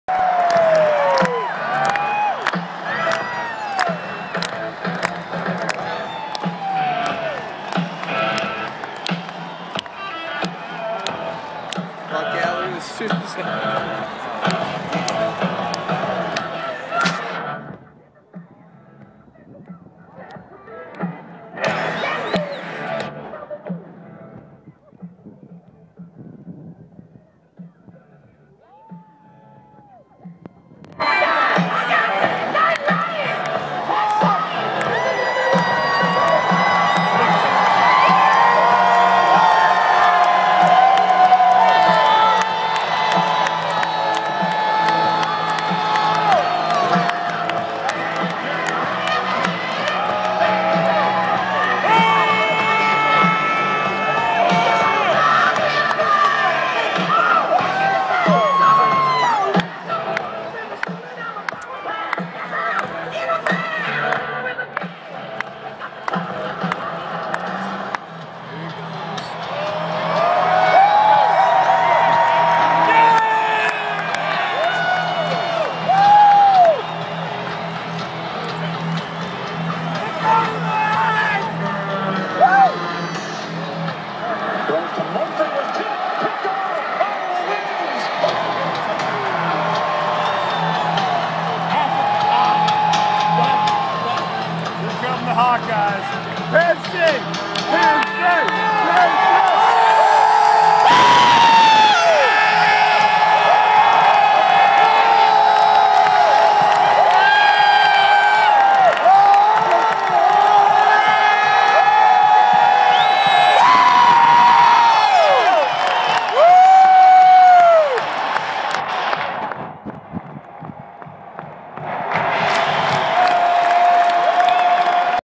Hawkeye Football Entrance
Vs. Penn State Oct. 20, 2012. The game was awesome...until kickoff. Brutal defeat.